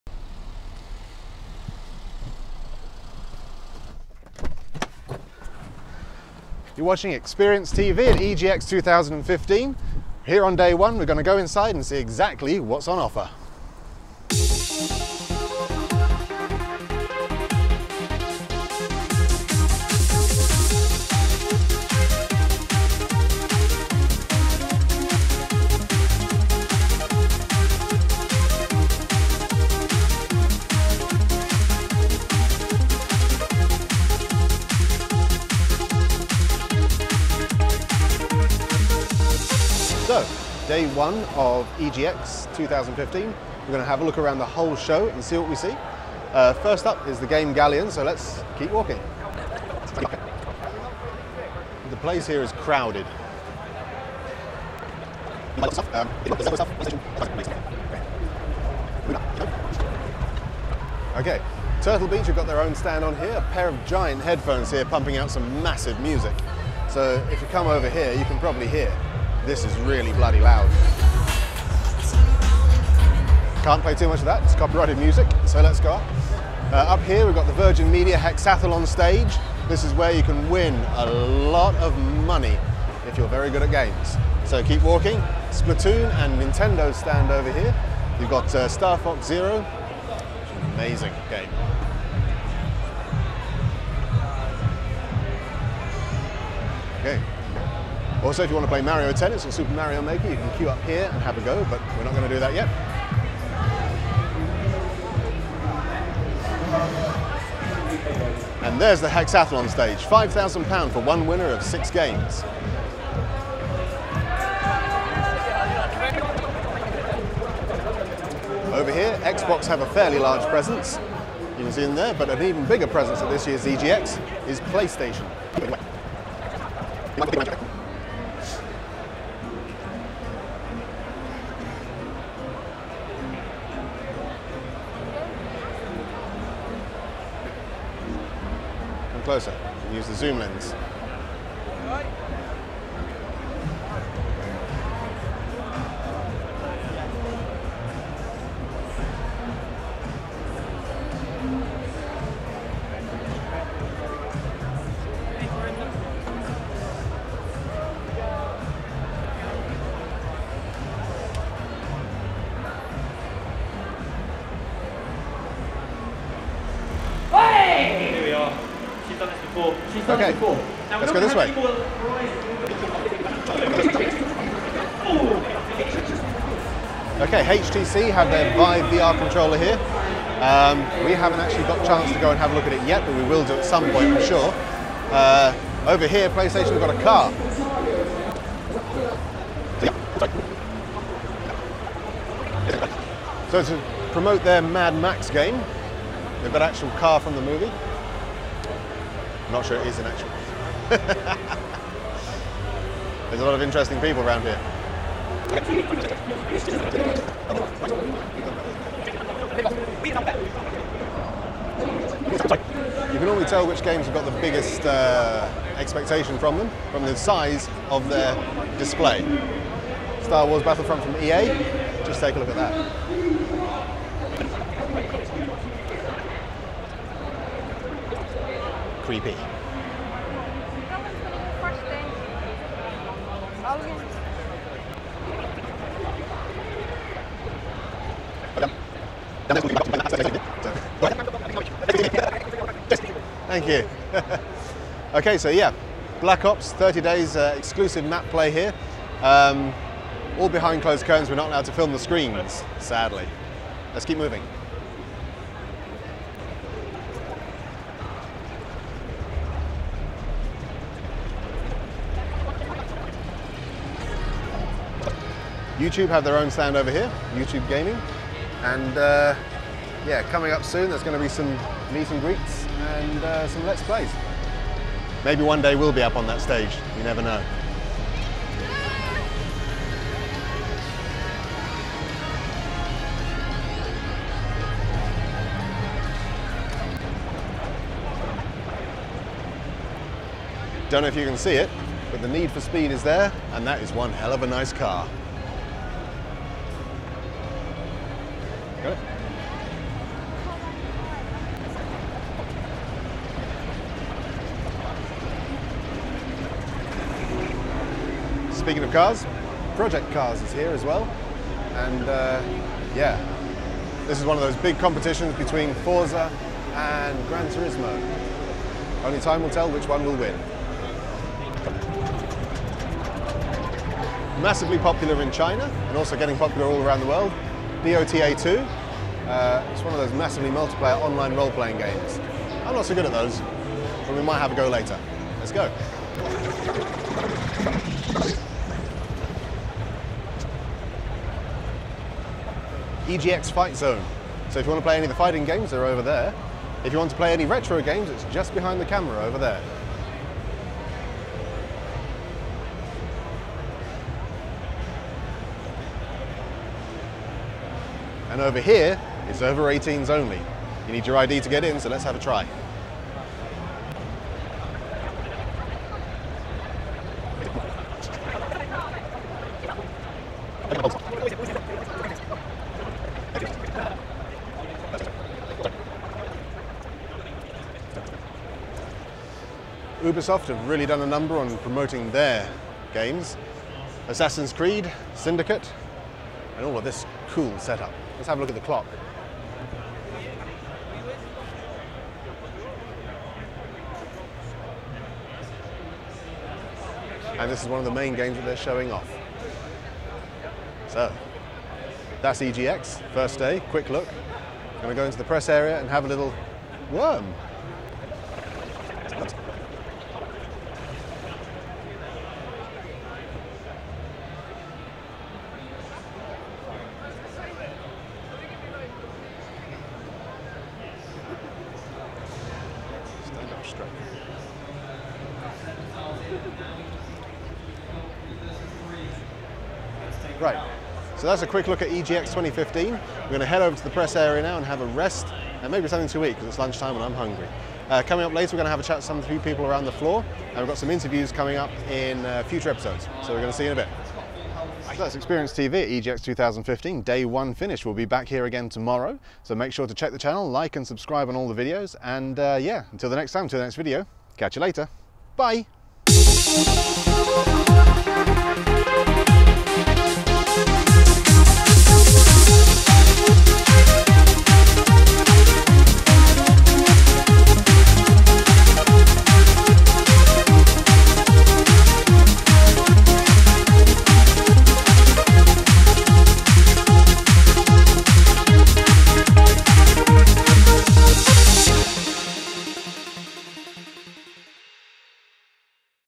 EGX 2015 - Day One Walkabout
Xperienced TV are at EGX 2015! We take a quick look around the show floor. Keep checking back for interviews and more footage from EGX over the coming week!

Camera Operator ► Shaochun Chen

Special Thanks ► EGX Organisers & Exhibitors